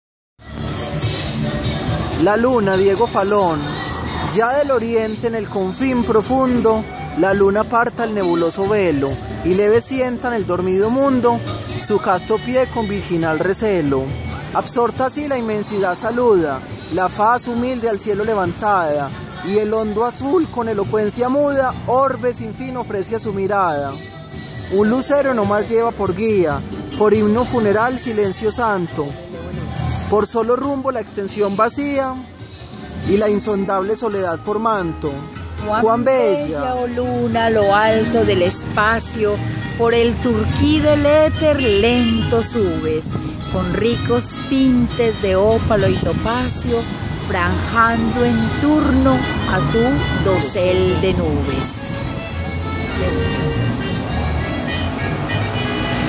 Poema a la Luna por Diego Fallon, en Copacabana
2 de mayo de 2015
Parte del poema a la Luna por Diego Fallon, en Copacabana
Lectores: Luis Fernando Gil y Margarita Moreno T.